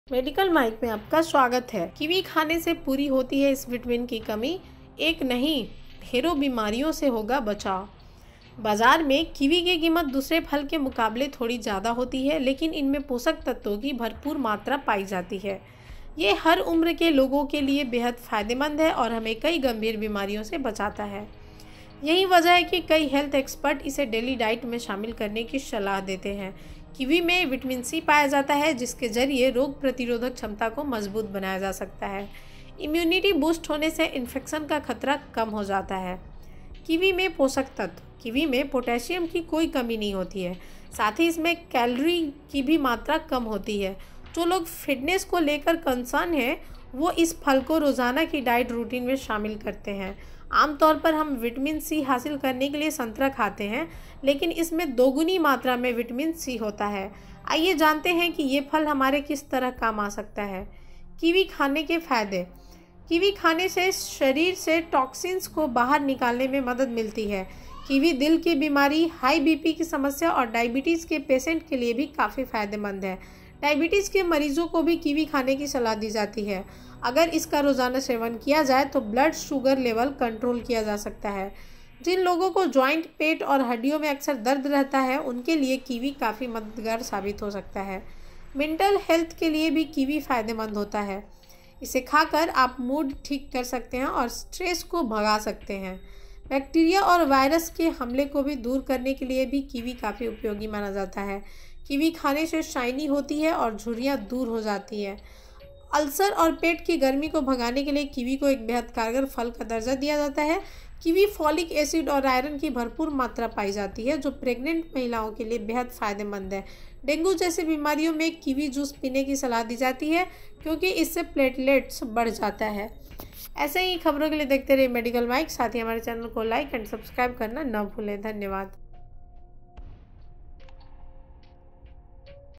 Kiwi खाने से पूरी होती है इस Vitamin की कमी, एक नहीं, ढेरों बीमारियों से होगा बचाव।
Kiwi खाने से पूरी होती है इस Vitamin की कमी, एक नहीं, ढेरों बीमारियों से होगा बचाव। बाजार में कीवी की कीमत दूसरे फल के मुकाबले थोड़ी ज्यादा होती है, लेकिन इनमें पोषक तत्वों की भरपूर मात्रा पाई जाती है। ये हर उम्र के लोगों को लिए बेहद फायदेमंद है और हमें कई गंभीर बीमारियों से बचाता है। यही वजह है कि कई हेल्थ एक्सपर्ट इसे डेली डाइट में शामिल करने की सलाह देते हैं। कीवी में विटामिन सी पाया जाता है जिसके जरिए रोग प्रतिरोधक क्षमता को मजबूत बनाया जा सकता है। इम्यूनिटी बूस्ट होने से इंफेक्शन का खतरा कम हो जाता है।